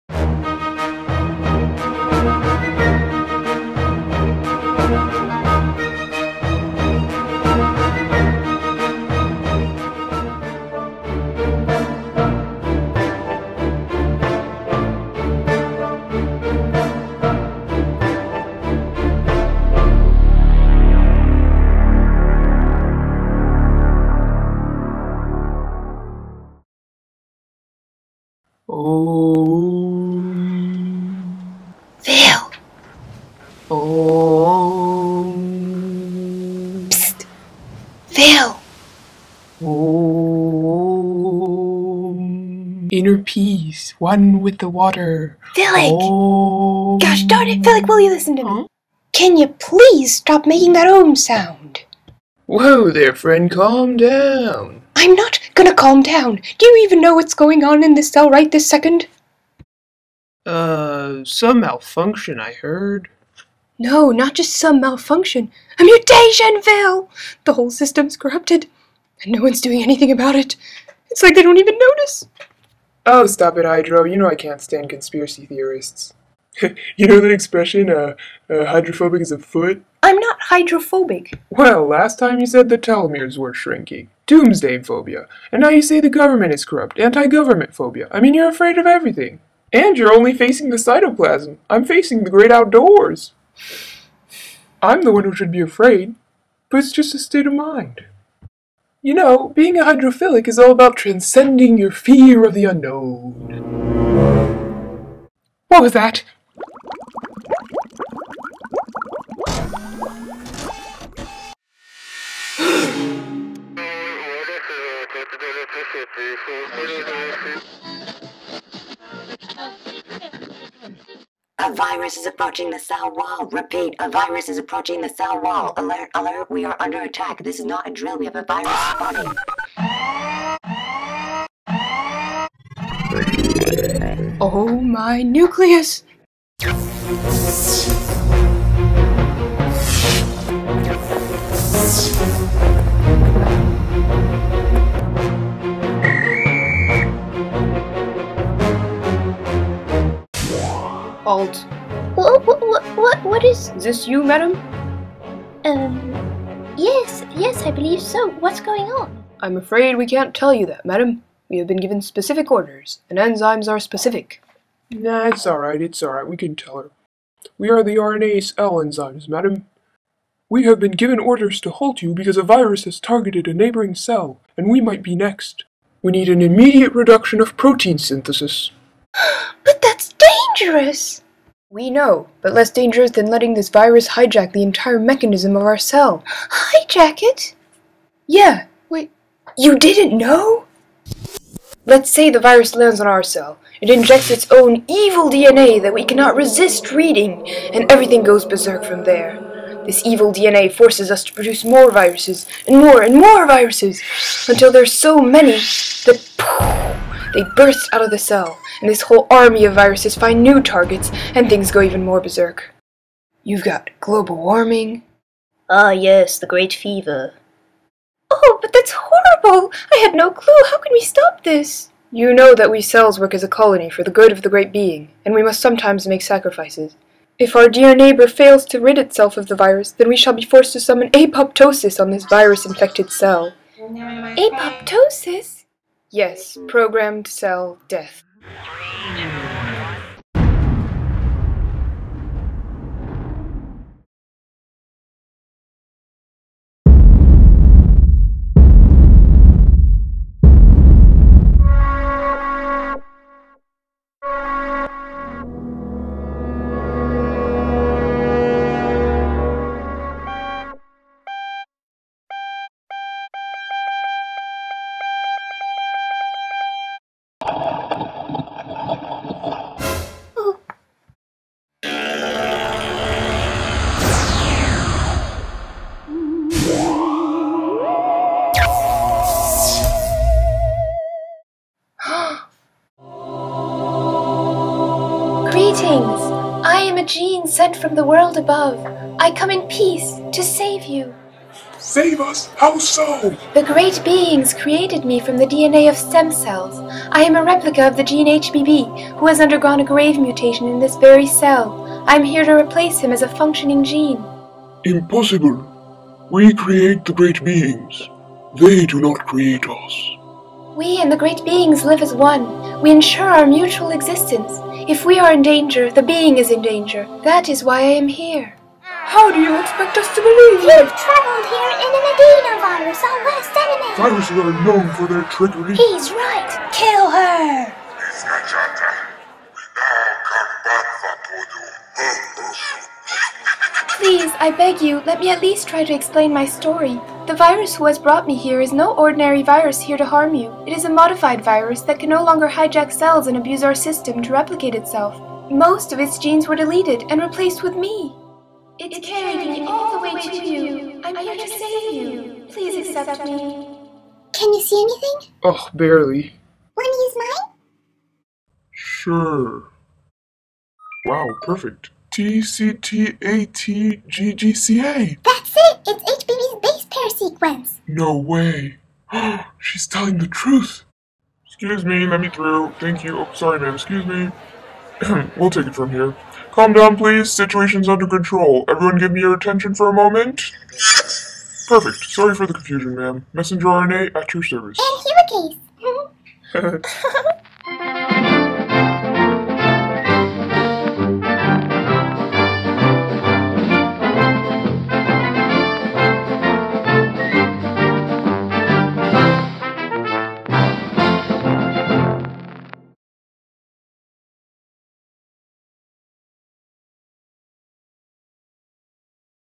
DNA Transcription: Lost in Translation, a Tiny Story of Gene Expression, Part 2
This is the second part of a short animation series on Sickle Cell Anemia, dedicated to kids who want to learn more about the role genes have in the body. While Part I explained the process of transcription and translation and the enormous impact that a single gene mutation can have on the body, Part II explains how gene therapy could be used to cure such a mutation...